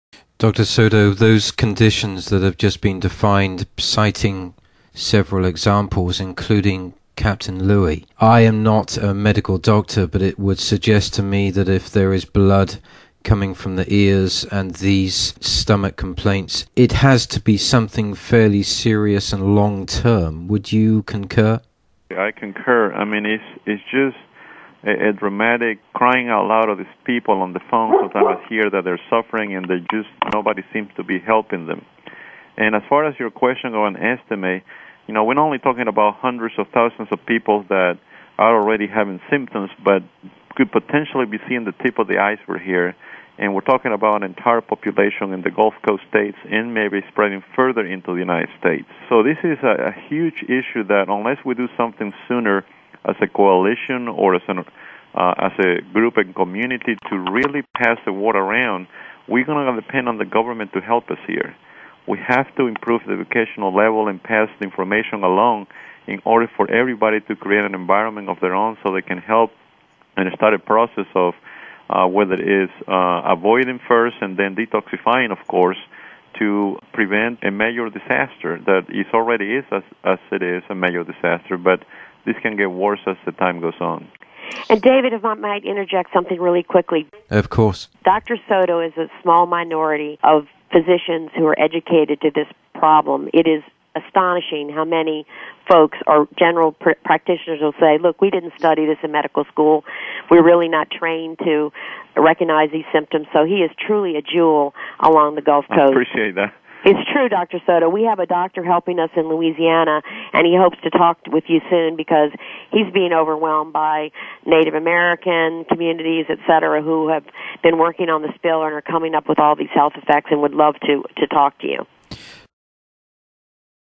100,000's having symptoms from oil disaster, could be just the tip of the iceberg says doctor
In Discussion with David Gibbons, February 25, 2011